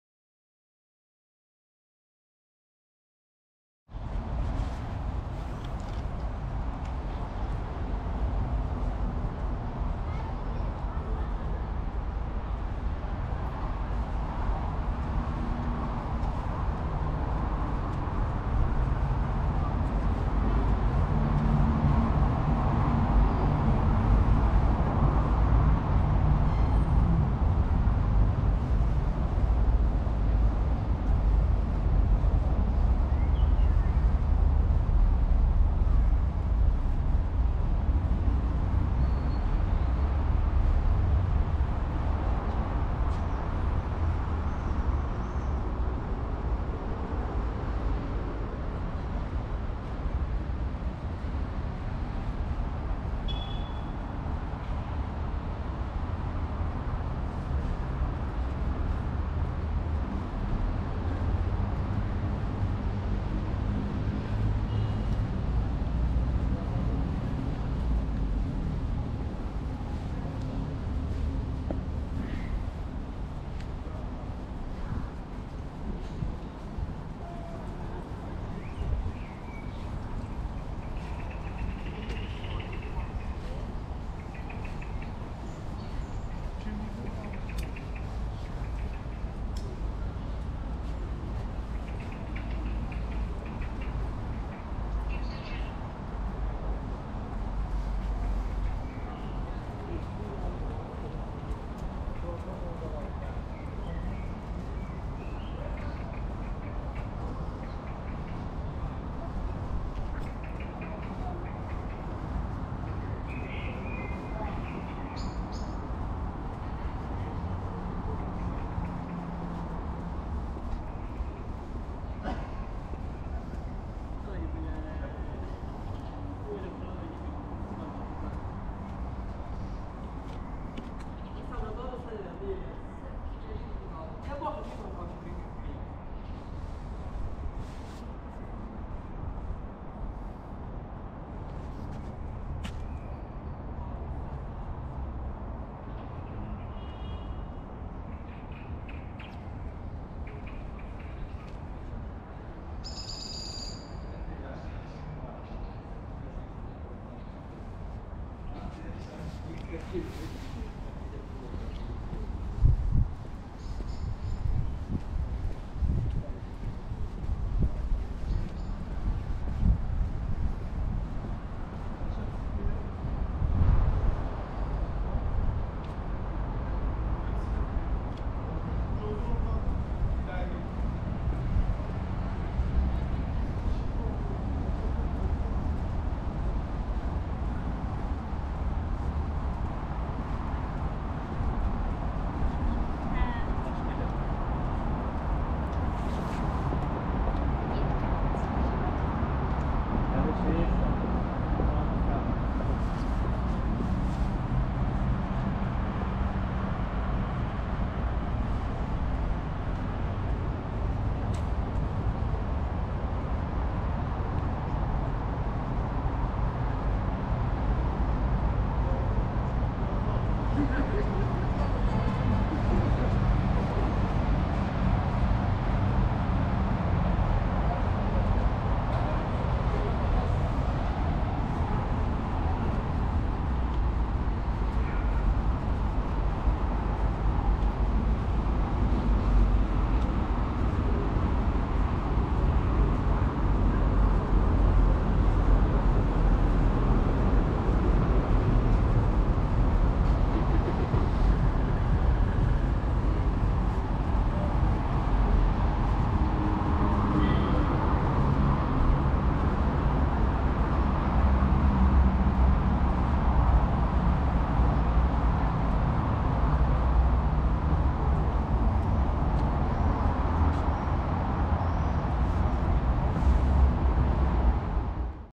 Гянджинская государственная филармония им. Фикрета Амирова
Ganja State Philharmonic Hall is a concert hall in Ganja, Azerbaijan. Ganja State Philharmonic is named after the composer Fikret Amirov.